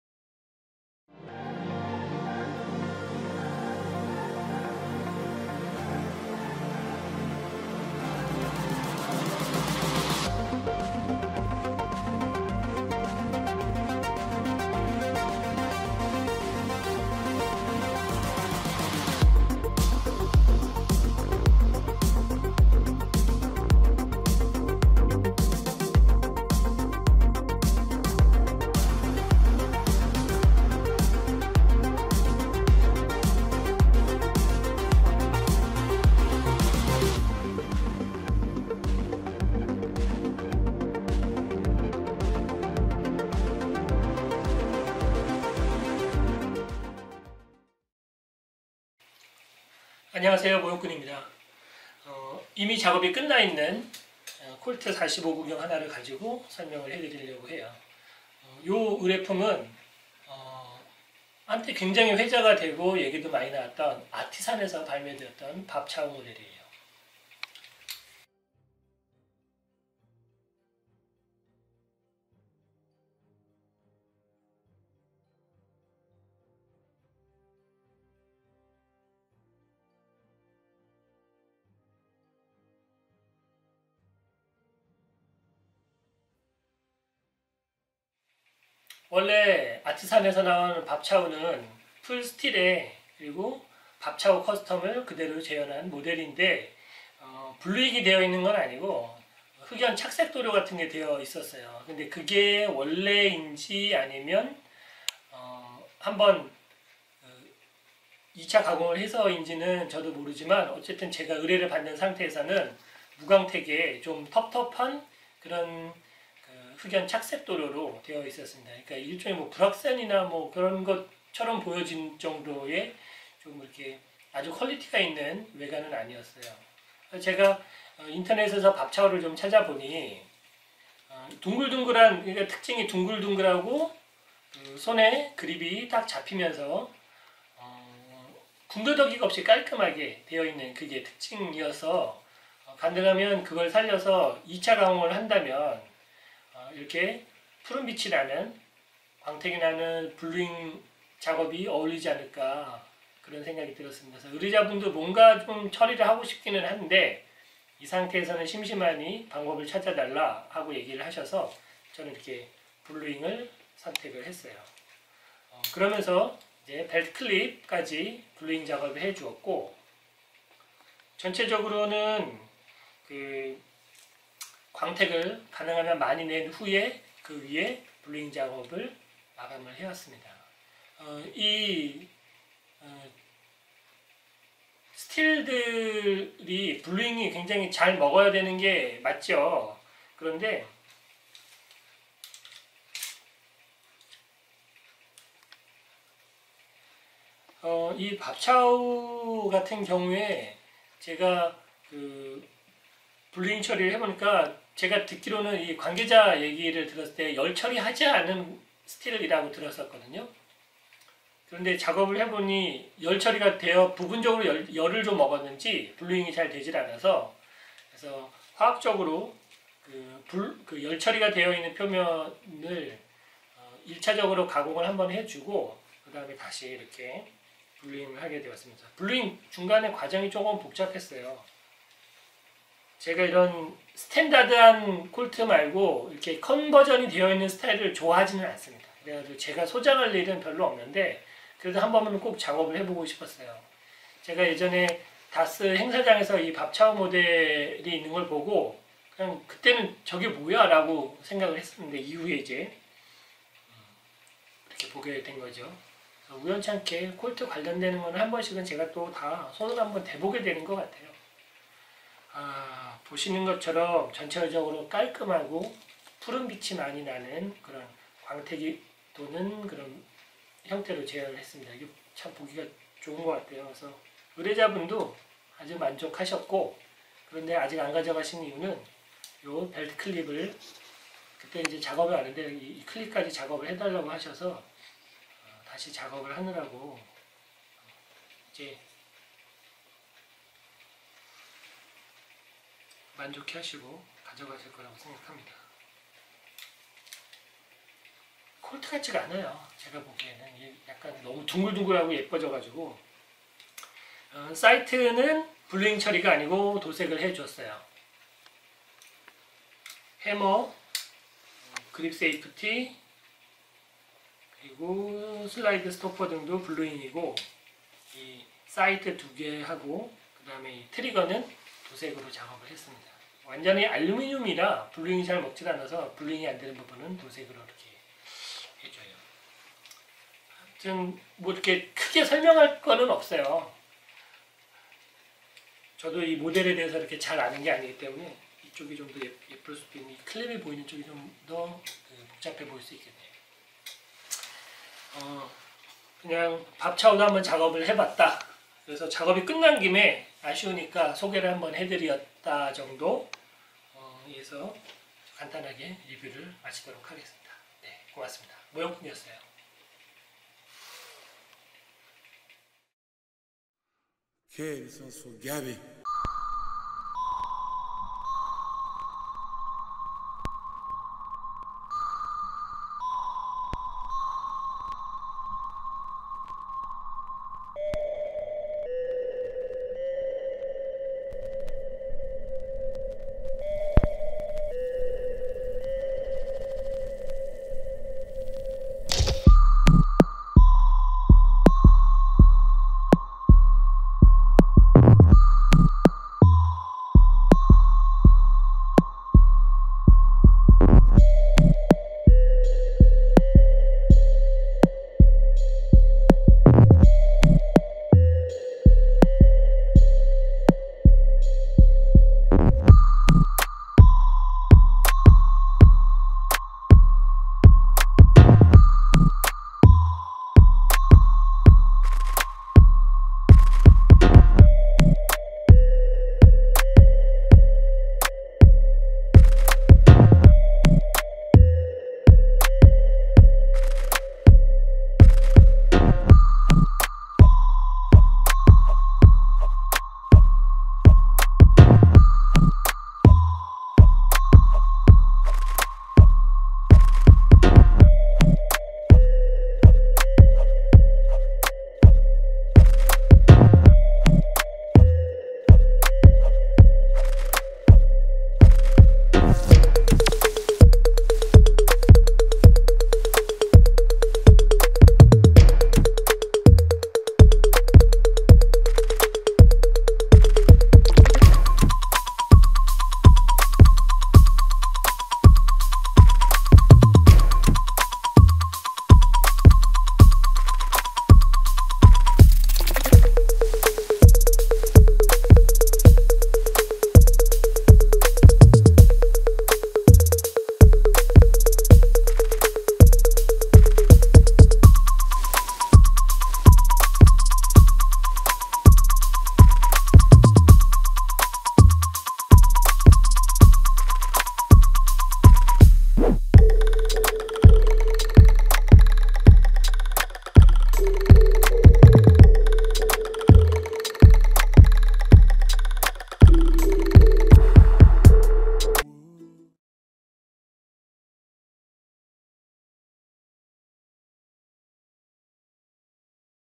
(의뢰작) 풀스틸 밥차우스페셜 1911 블루잉 완성작
킷만 350만원 짜리인 아티산에서 재현한  밥차우 모델을
모형꾼 스타일의 블루잉으로 완성하여 소개해드립니다.